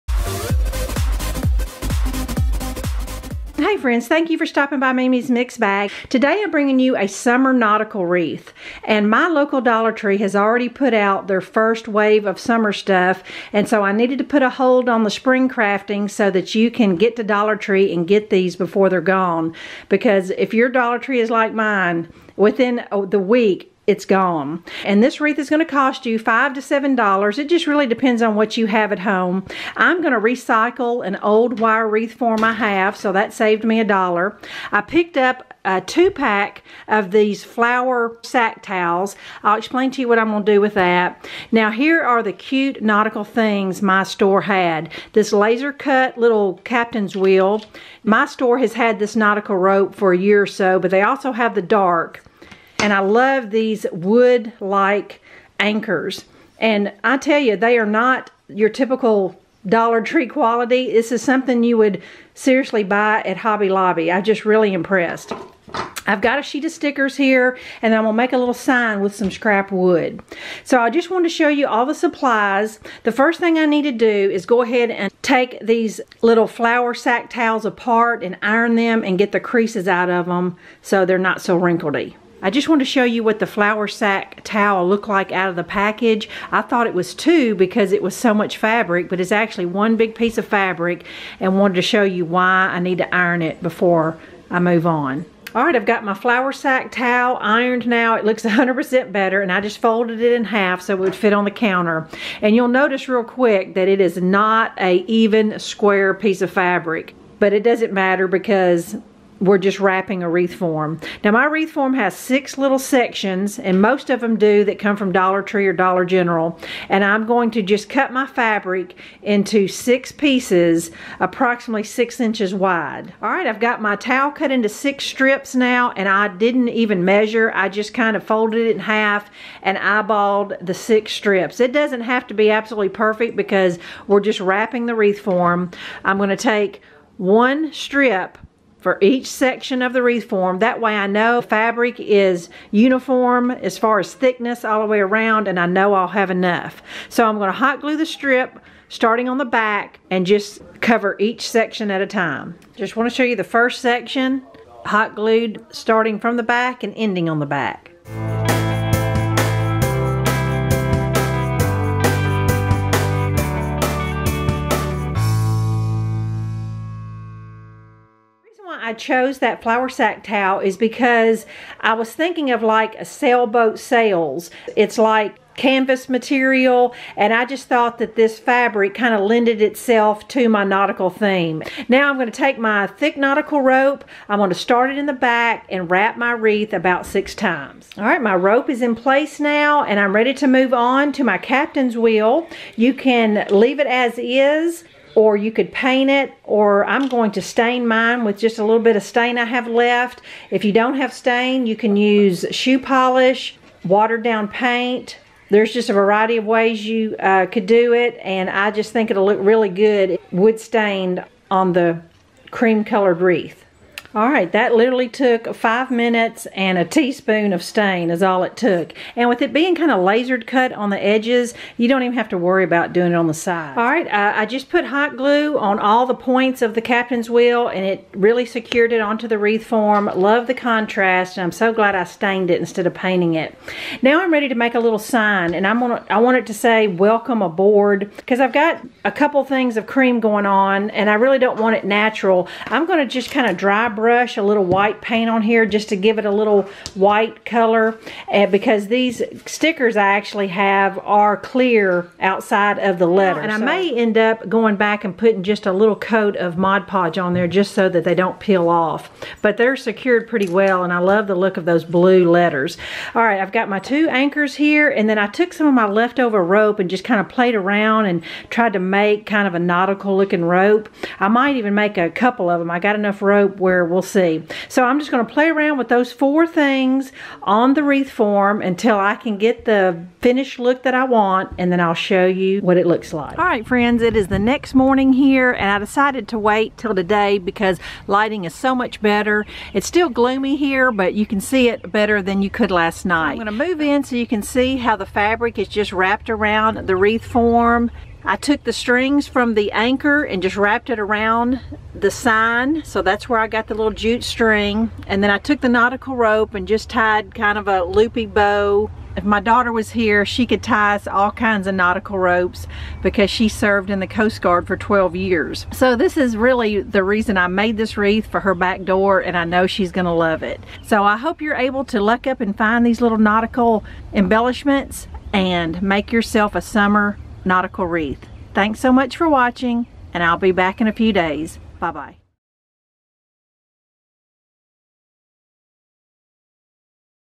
Dollar Tree Nautical Summer Wreath
Create  and adorable nautical themed wreath from Dollar Tree Summer 2021 collection.  Perfect gift for a military person or boat owner's front door.

🗄REFERRAL & Discount  LINKS:
(not a scam at all!!!)